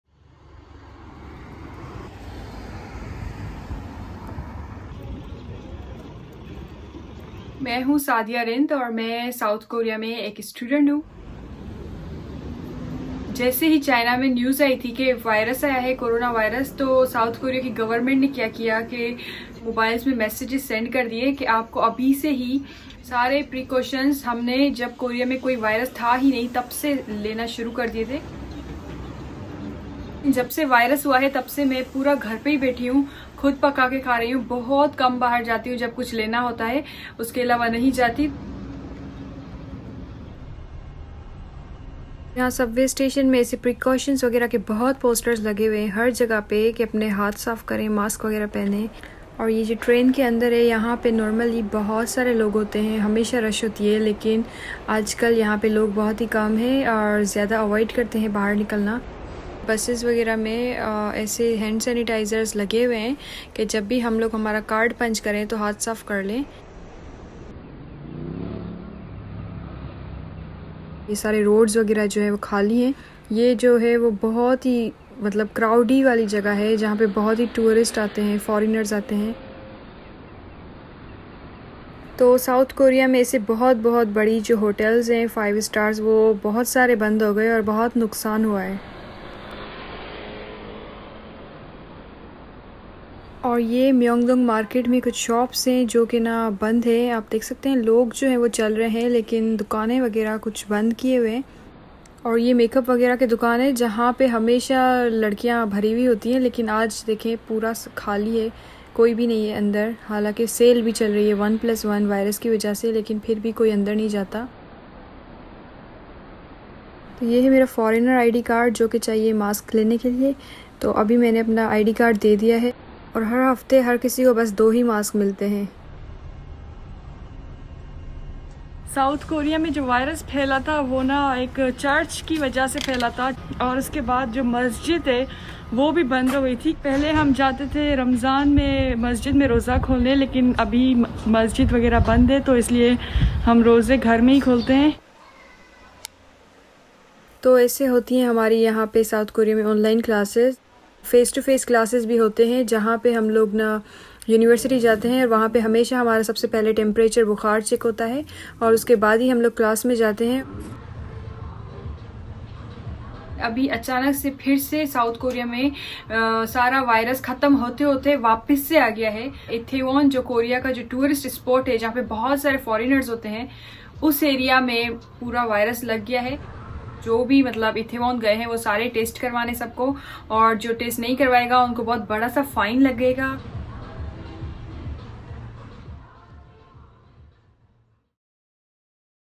Pakistani student in South Korea: "I am only allowed to buy two masks a week!" - BBC URDU
مشرقِ بعید کے ملک جنوبی کوریا میں اگرچہ کورونا وائرس کے متاثرین کی تعداد نسبتاً کم ہے لیکن وہاں بھی اِس وبا سے بچاؤ کے لیے سخت احتیاطتی تدابیر اختیار کی جا رہی ہیں۔
دارالحکومت سیول میں کیا حالات ہیں، بتا رہی ہیں وہاں موجود پاکستانی طالبہ سعدیہ رند اپنے اِس وی لاگ میں۔